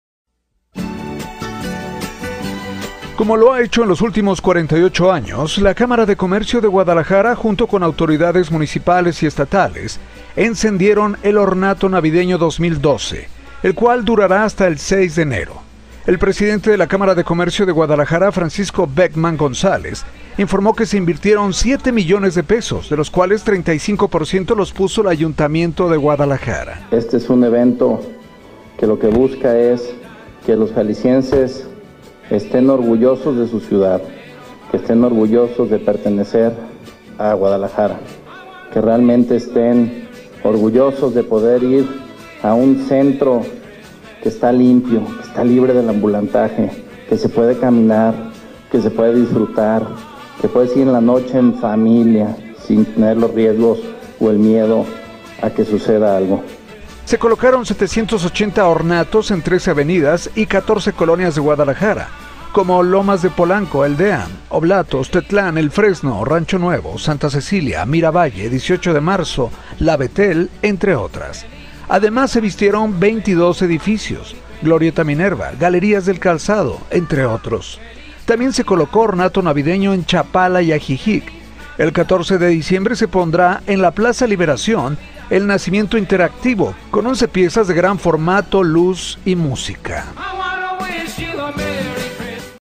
Iluminan la ciudad para la temporada navideña 2012
La Cámara de Comercio de Guadalajara, autoridades municipales y estatales encendieron el Ornato Navideño 2012 que durará hasta el 6 de enero